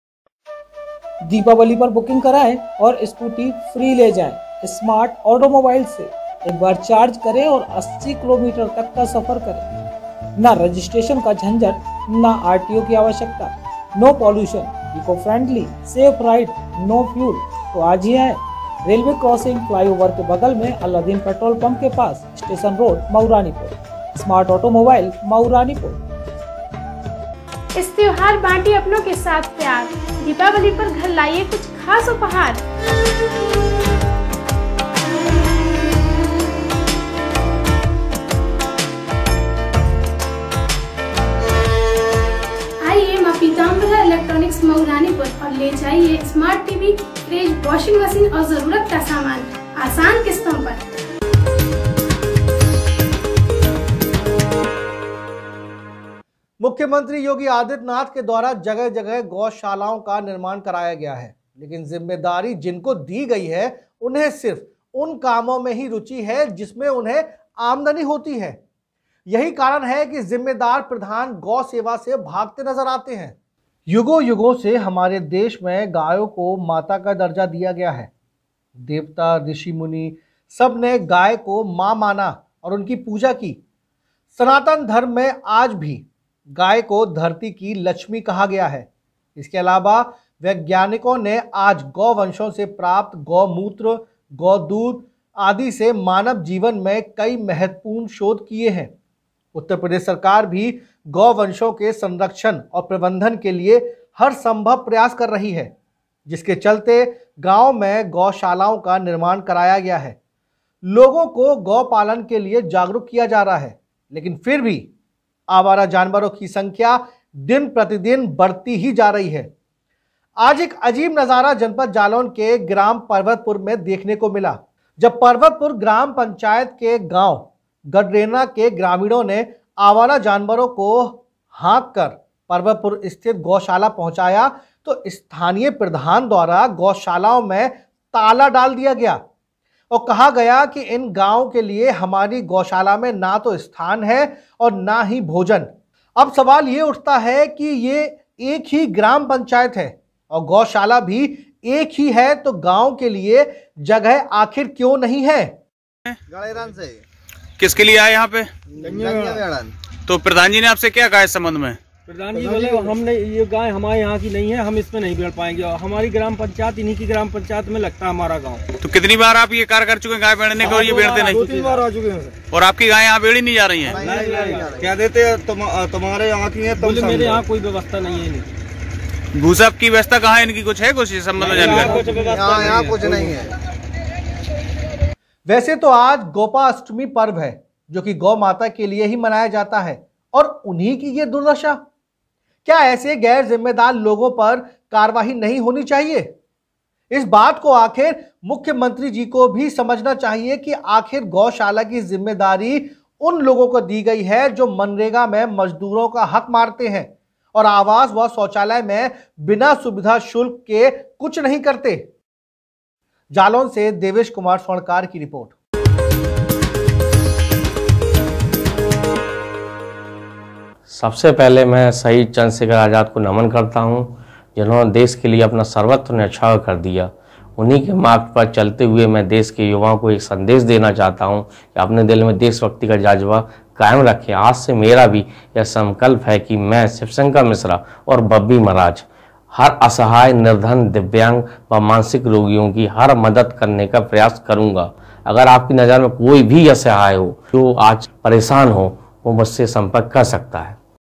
जालौन में प्रधान की ऐसी दबंगई, बोला नहीं घुसने दूंगा गायों को गौशाला में।
Report : Devesh Kumar